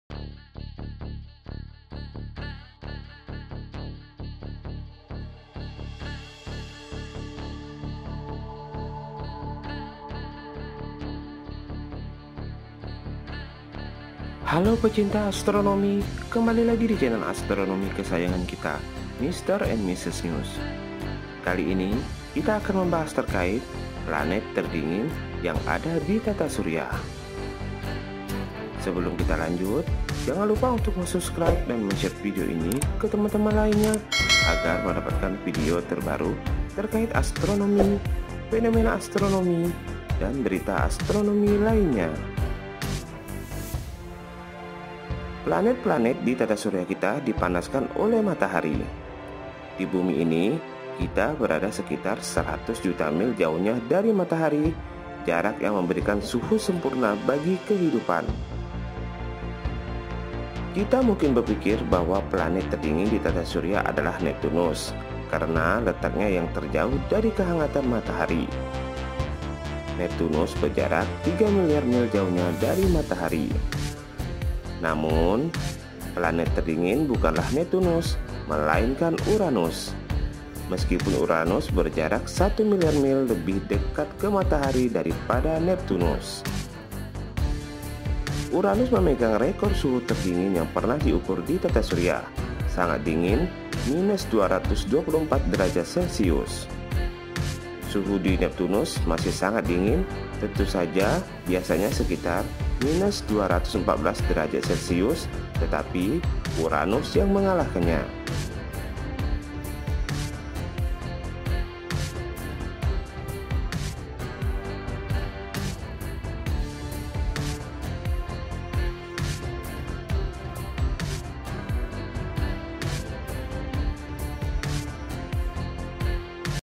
PLANET TERDINGIN DI TATA SURYA, PLANET URANUS
PLANET TERDINGIN
THE COLDEST PLANET
THE COLDEST PLANET IN THE SOLAR SYSTEM
PLANET TERDINGIN DI TATA SURYA

Planet-planet di Tata Surya kita dipanaskan oleh Matahari. Di Bumi ini, kita berada sekitar 100 juta mil jauhnya dari Matahari – jarak yang memberikan suhu sempurna bagi kehidupan. KITA mungkin berpikir bahwa planet terdingin di Tata Surya adalah Neptunus, karena letaknya yang terjauh dari kehangatan Matahari. Neptunus berjarak tiga miliar mil jauhnya dari Matahari.Namun, planet terdingin bukanlah Neptunus, melainkan Uranus – meskipun Uranus berjarak satu miliar mil lebih dekat ke Matahari daripada Neptunus. Uranus memegang rekor suhu terdingin yang pernah diukur di Tata Surya: sangat dingin -224℃. Suhu di Neptunus masih sangat dingin, tentu saja – biasanya sekitar -214℃ – tetapi Uranus mengalahkannya. Miliaran tahun yang lalu asteroid yang sangat besar menabrak Uranus yang menyebabkannya terguling ke samping. Tabrakan tersebut menyebabkan panas dalam Uranus terlepas dan menjadikannya planet yang sangat dingin.Panas di dalam Uranus merupakan sisa-sisa panas yang tercipta saat planet ini terbentuk. Pembentukan planet ini terjadi karena bongkahan batu kecil saling bertabrakan yang membentuk seluruh planet selama jutaan tahun. Pada saat batu-batu ini saling bertabrakan, mereka akan mendapatkan sedikit lebih banyak panas yang disimpan untuk menjaga suhu dari planet tersebut. Sayangnya, asteroid yang sangat besar menabrak Uranus yang menyebabkan planet ini melepaskan seluruh panasnya dan menyebabkan suhu planet ini menjadi sangat dingin. Berbeda dengan Uranus, Neptunus tidak pernah ditabrak oleh asteroid yang menyebabkannya tetap dapat mempertahankan panasnya. Kendati demikian, suhu di Neptunus tetap sangat dingin, yaitu sekitar -214 derajat Celcius.